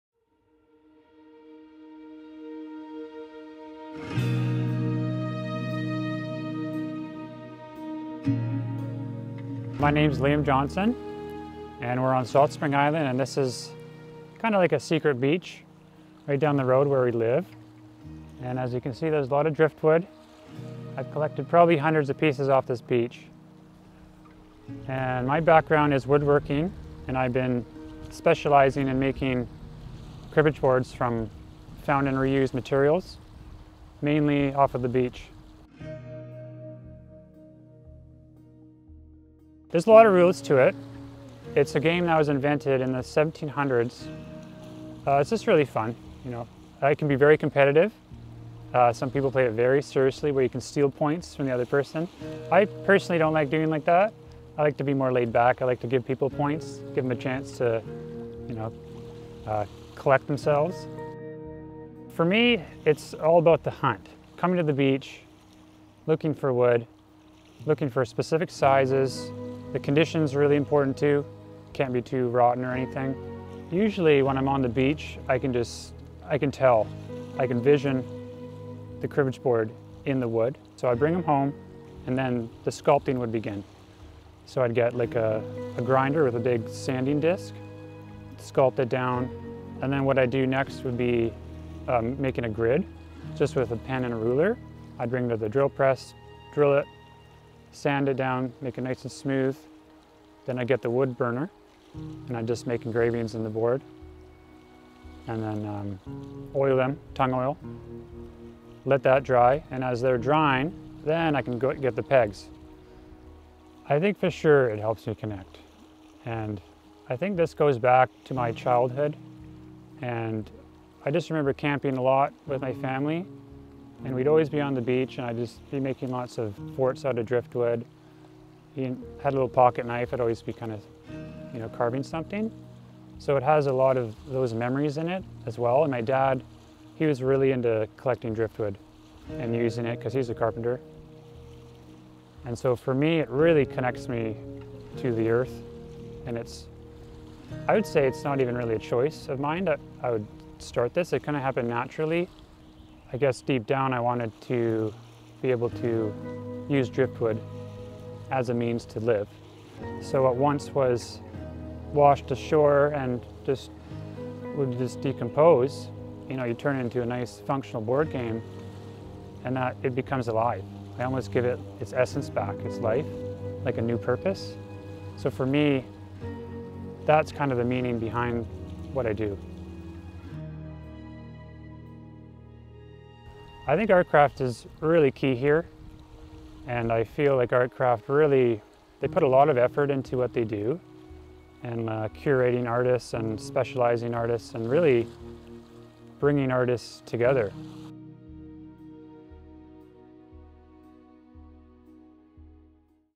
Salt Spring Arts - Liam Johnson, Gulf Island Cribbage
An interview with Artcraft artist Liam Johnson, creator of Gulf Island Cribbage.